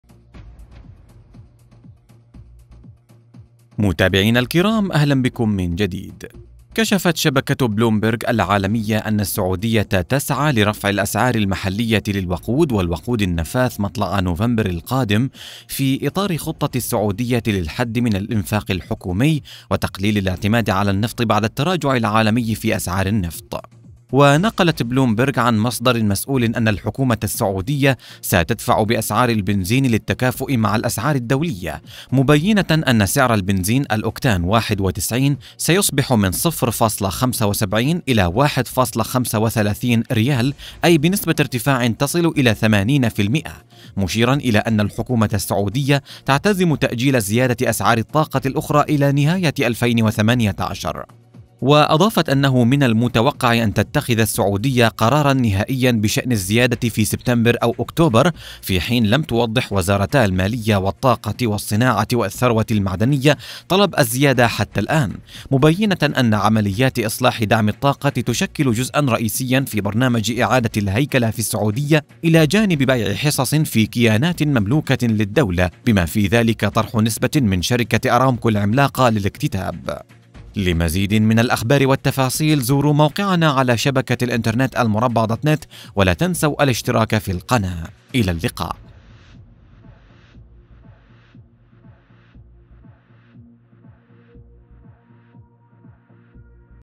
تقارير تشير الى ارتفاع اسعار البنزين 80% في السعودية
المربع نت - كشفت شبكة بلومبيرج العالمية أن السعودية تسعى لرفع الأسعار المحلية للوقود والوقود النفاث مطلع نوفمبر القادم شاهد التفاصيل والصور بالكامل: